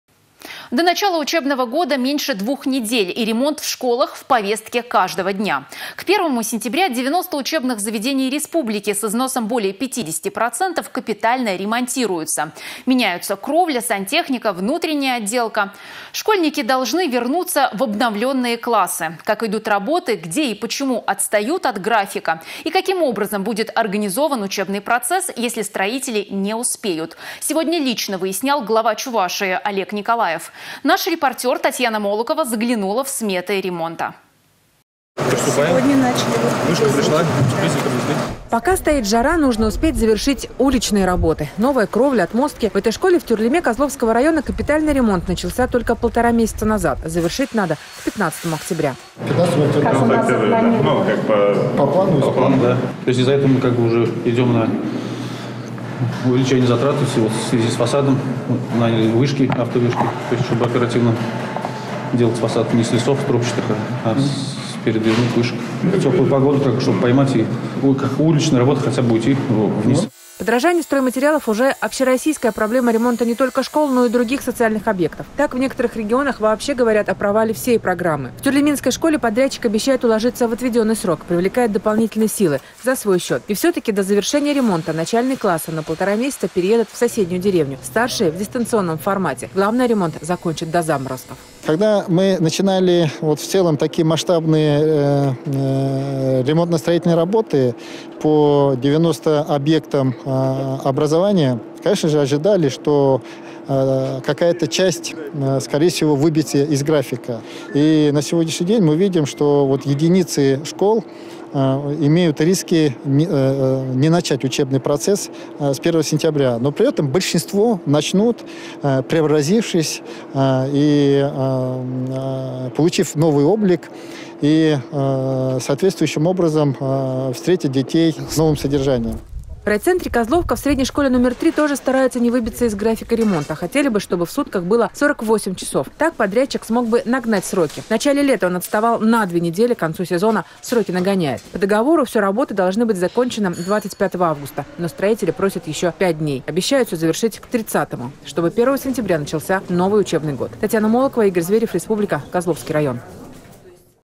Ремонт в некоторых школах Чувашии не закончат ко дню знаний
До начала учебного года меньше двух недель и ремонт в школах – в повестке каждого дня. К первому сентября 90 учебных заведений республики, с износом более 50%, капитально ремонтируются: меняется кровля, сантехника, внутренняя отделка. Школьники должны вернутся в обновленные классы. Будет меняться и начинка учебных аудиторий. Как идут работы, где и почему отстают от графика и каким образом будет организован учебный процесс, если строители не успеют сегодня лично выяснял Глава Чувашии Олег Николаев. Наш репортер Татьяна Молокова заглянула в сметы ремонта.